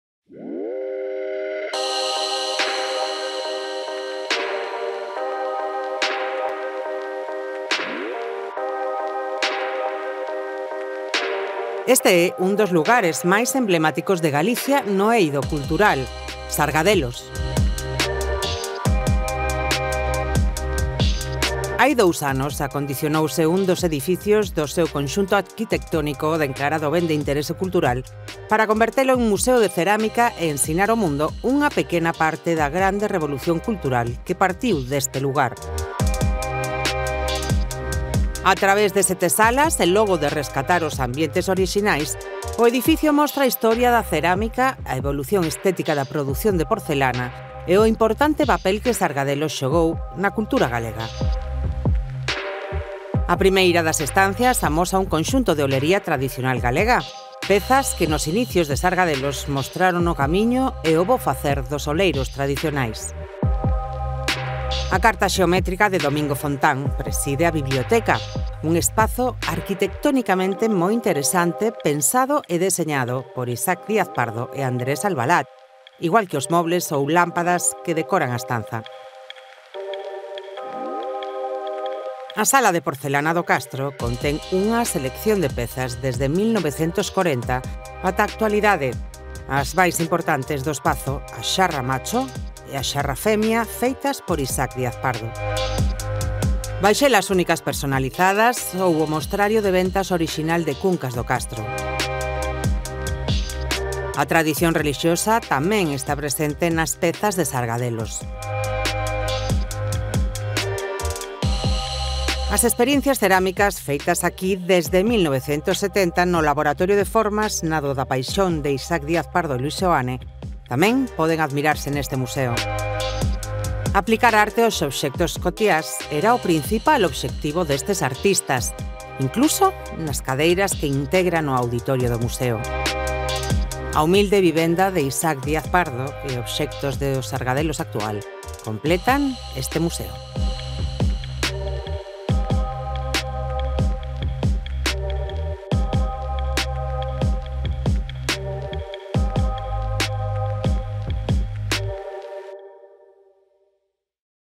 Descubrindo Lugo: Museo Sargadelos
Hoxe propoñémosvos unha interesante visita ao Museo de Sargadelos, no Concello de Cervo. Un percorrido pola historia da emblemática cerámica, que é todo un referente da cultura galega.